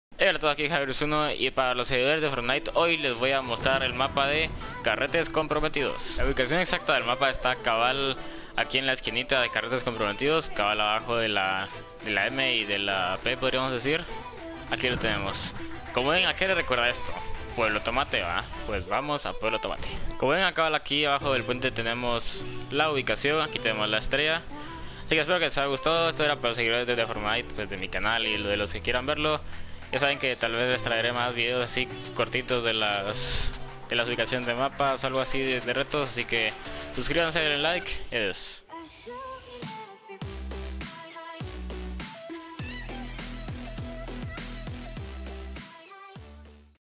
MAPA DEL TESORO DE CARRETES COMPROMETIDOS!
Hoy para los interesados en la nueva temporada 5 les traigo uno de los retos que es el del mapa del tesoro de carretes espero les guste y si es asi un like se agradece!
Opinen en los comentarios!!!